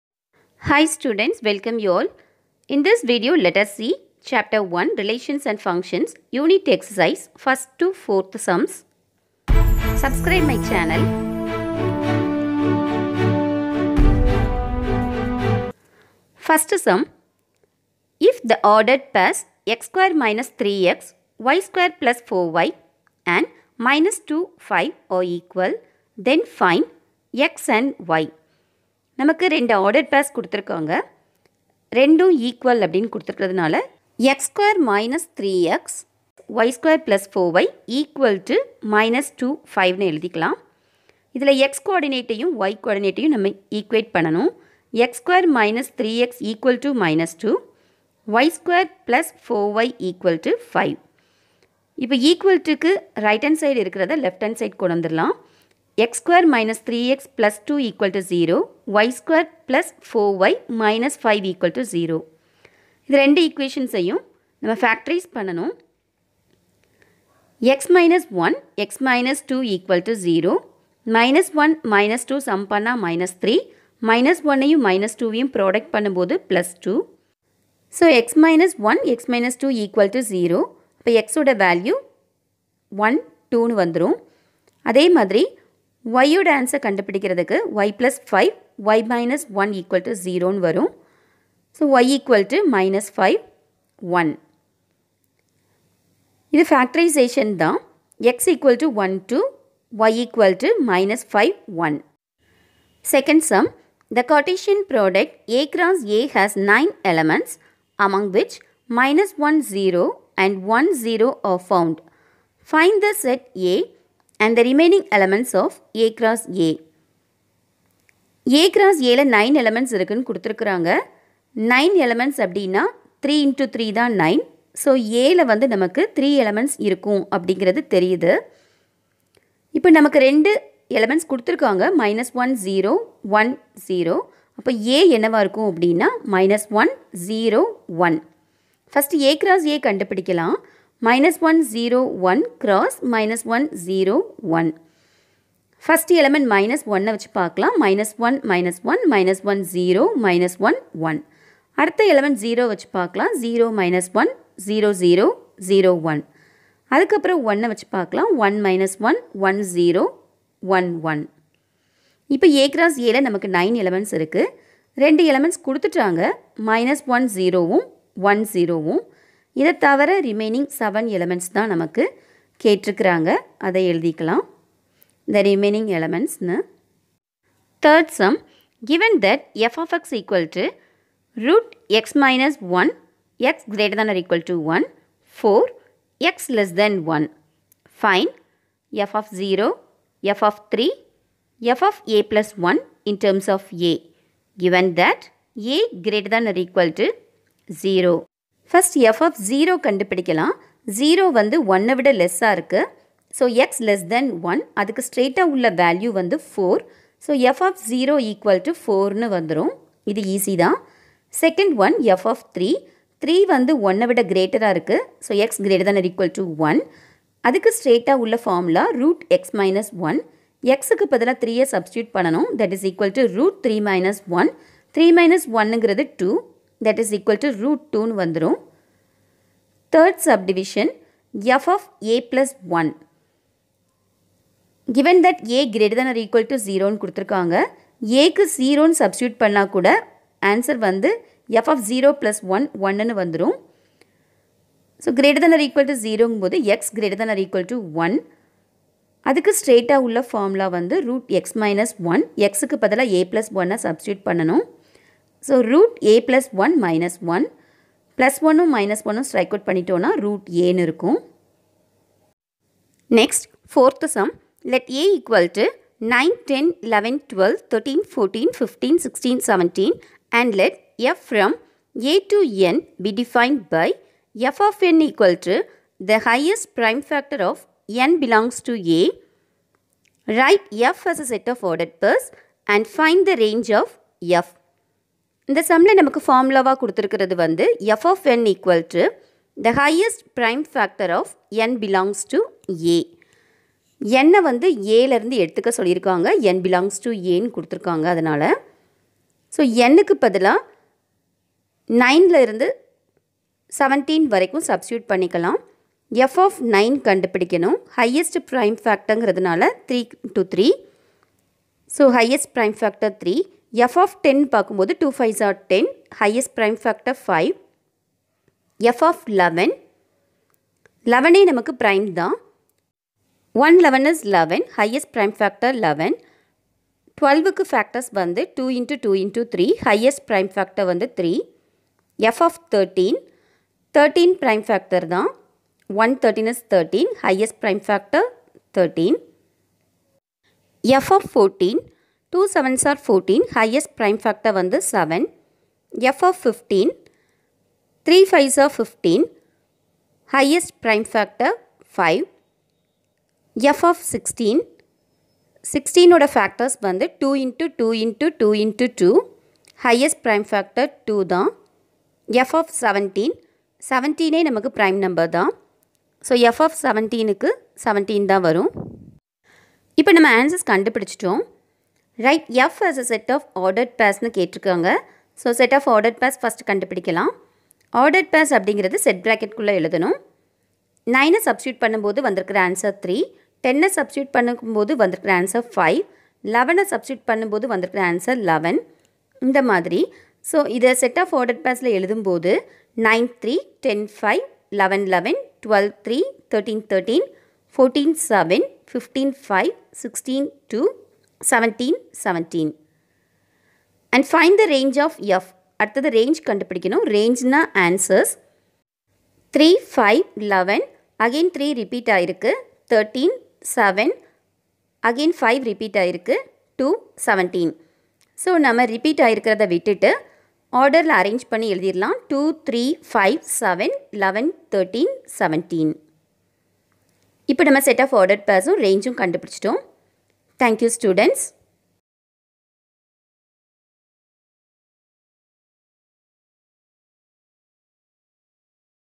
Eng Medium Std 10 Chap 1 Unit Ex Sums(1-4)
All sums are clearly explained with Animations. Students understand easily.